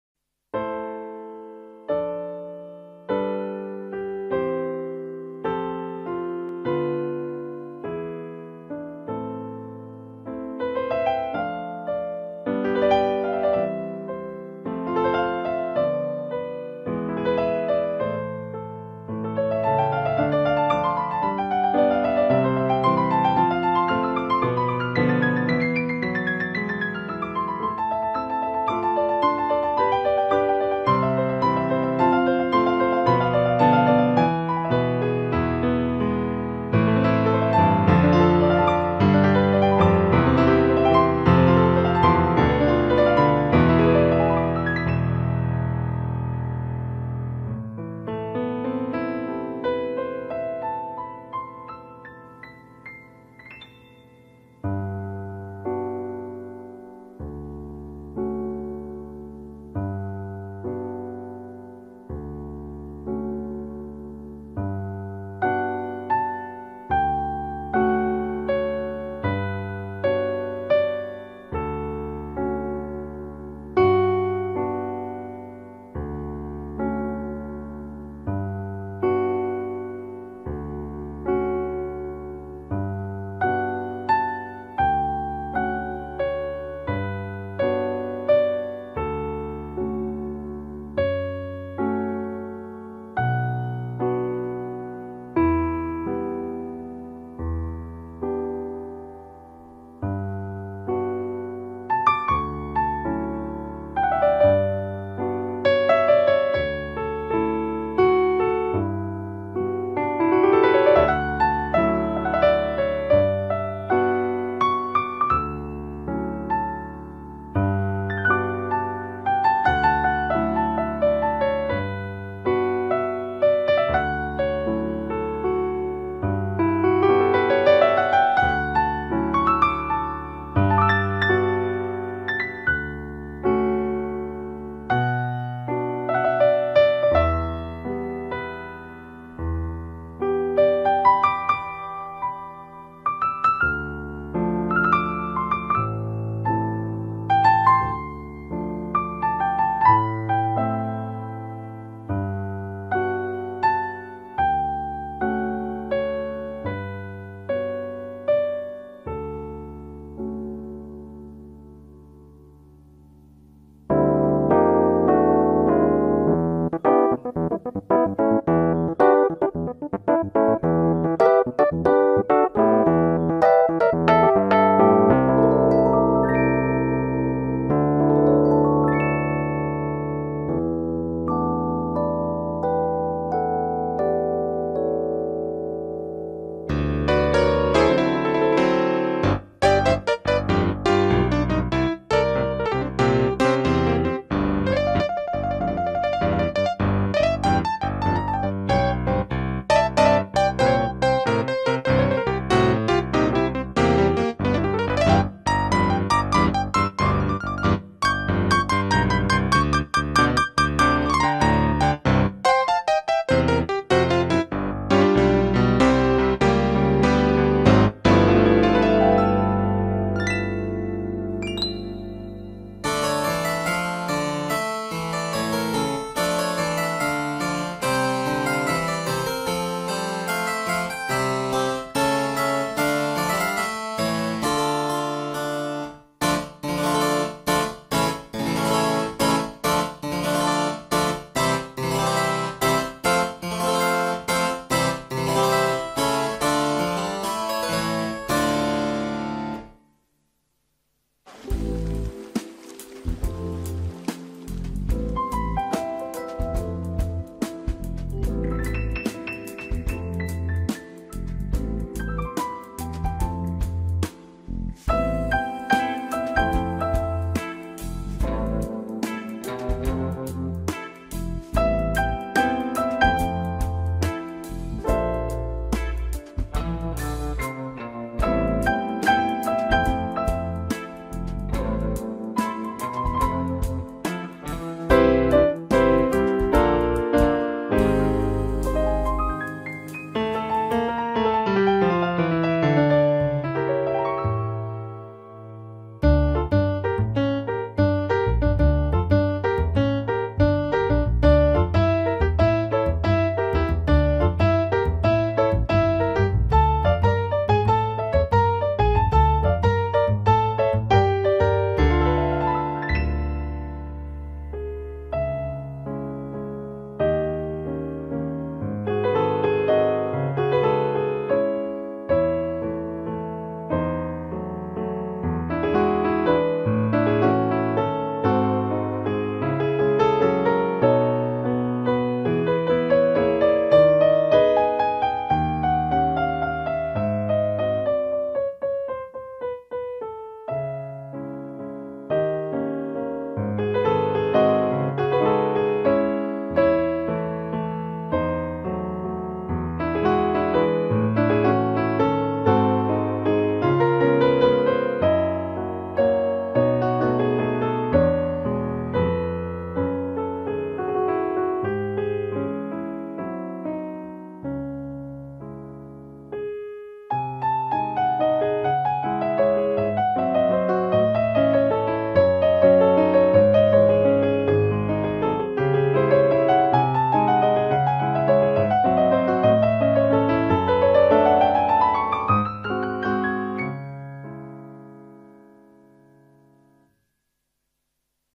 KAWAI CN39 Digital Piano DEMO Playing only
Kawai CN39: große Klangbibliothek, großartige Tastatur mit Druckpunkt in Kombination mit feinsten Audiokomponenten